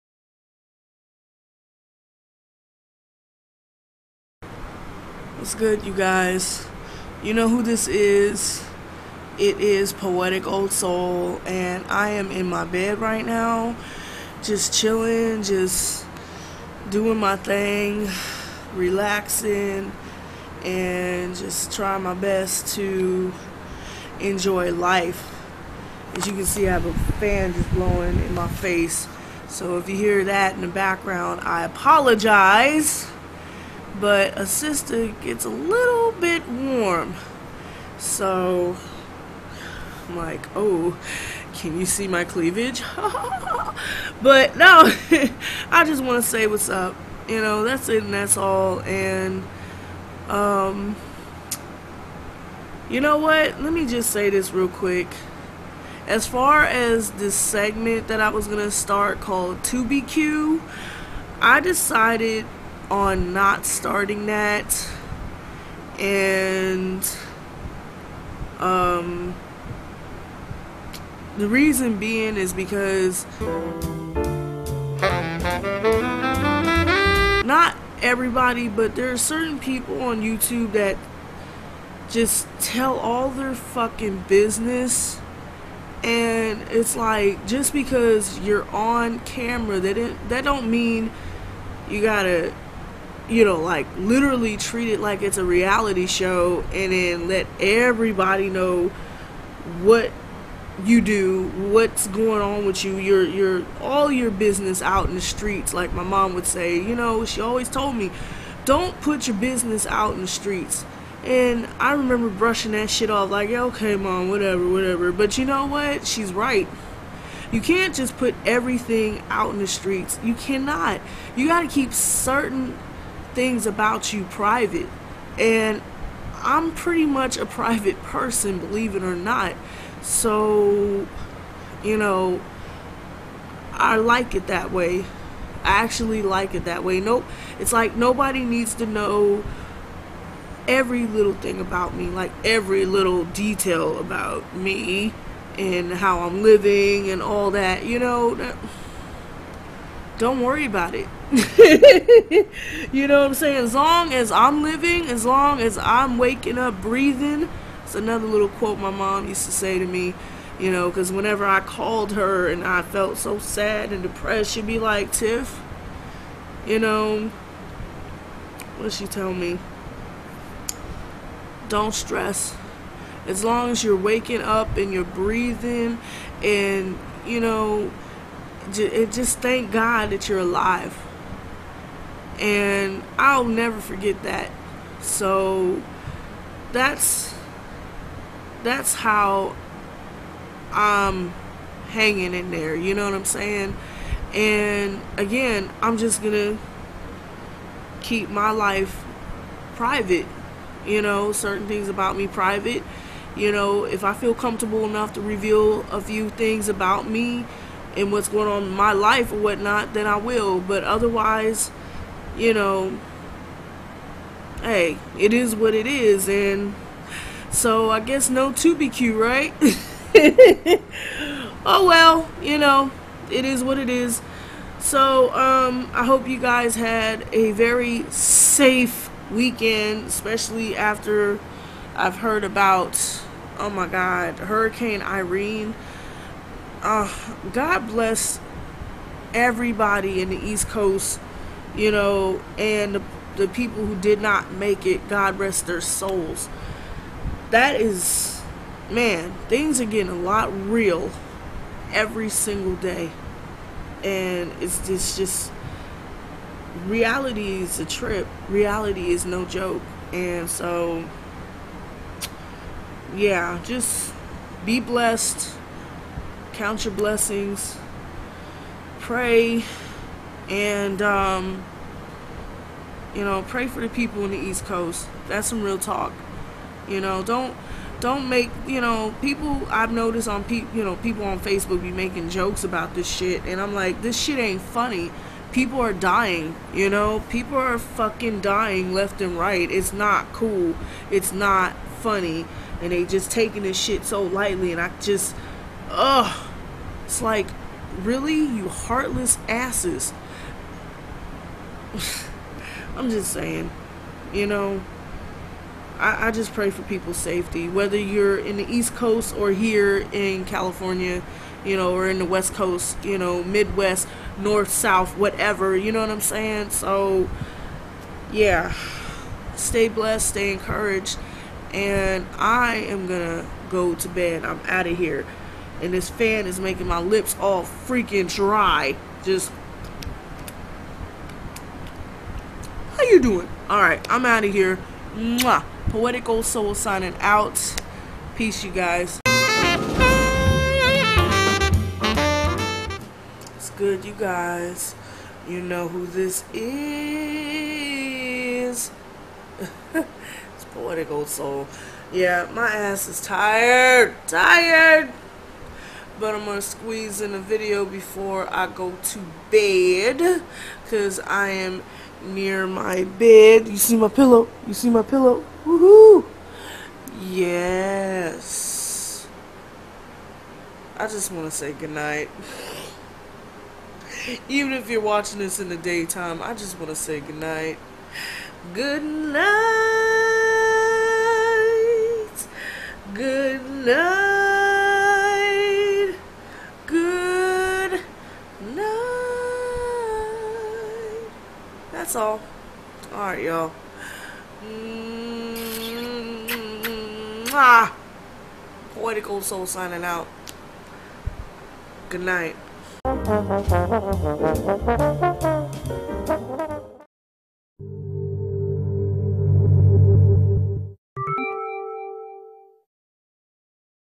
"Don't put your buisness out in the streets"
My reason for not wanting to start "Tubie Q"...yeah, just watch & listen ;) .

In case you're wondering what "Tubie Q" was (the "Q" means Questions and Tubie...yeah, ya'll know where that name is from LOL)...well, it was going to be a segment of me answering questions from YOU about Ms. Poetic Old Soul, and who would've thought that my subbies would actually ask me questions when I said "ask me anything"...OOPS...my bad LOL.